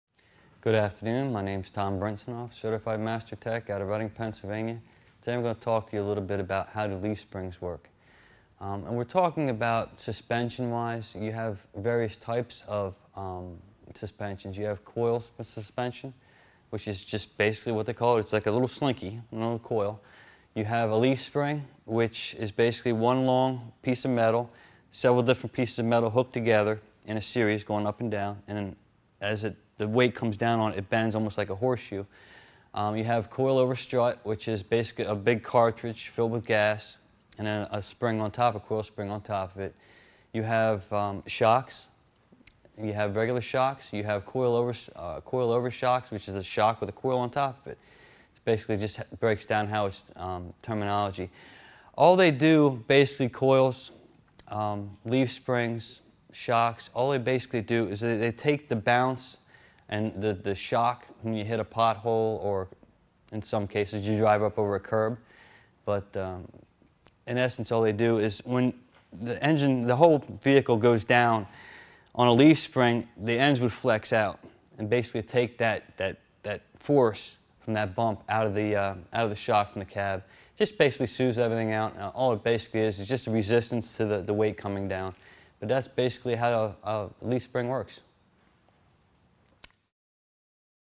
How Do Leaf Springs Work?
How Do Leaf Springs Work?. Part of the series: Auto Repair & Maintenance. Leaf springs are a type of suspension system for a car, much like shocks, and they are designed to take the bounce of a curb or a pothole. Find out how the force of a bump is smoothed out by leaf springs with help from an ASE-certified mechanic in this free video on auto parts and how they work.